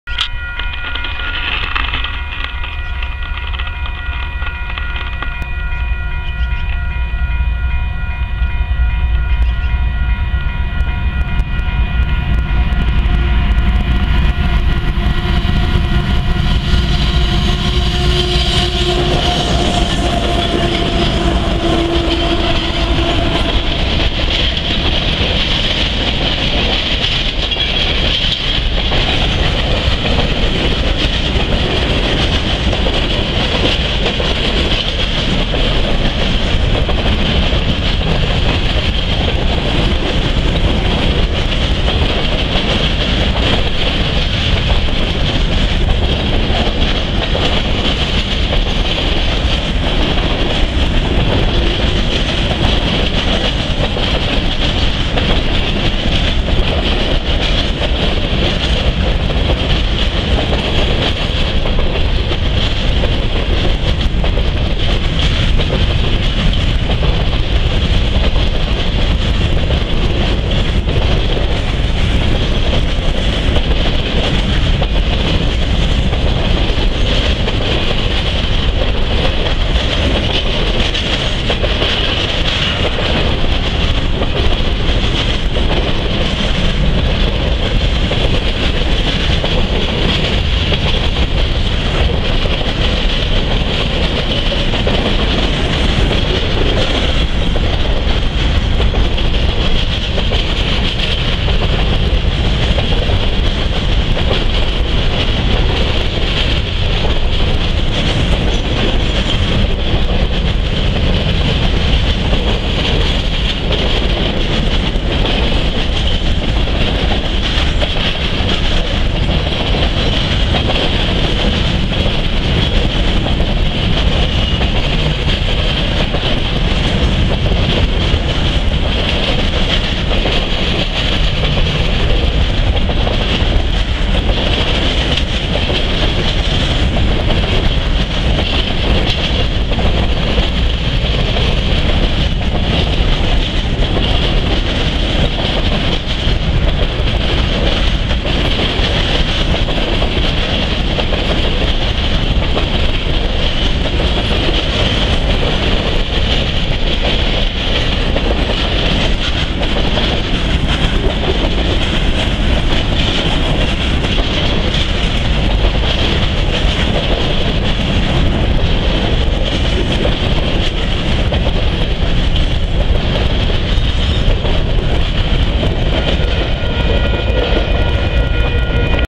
CP Rail * 5 * Mar. 26 2013,Edworthy Park,Calgary,Grain Train
Inbound Grain with Locos CP 8839,CP 8723
Soak Relfy1 2Face ect.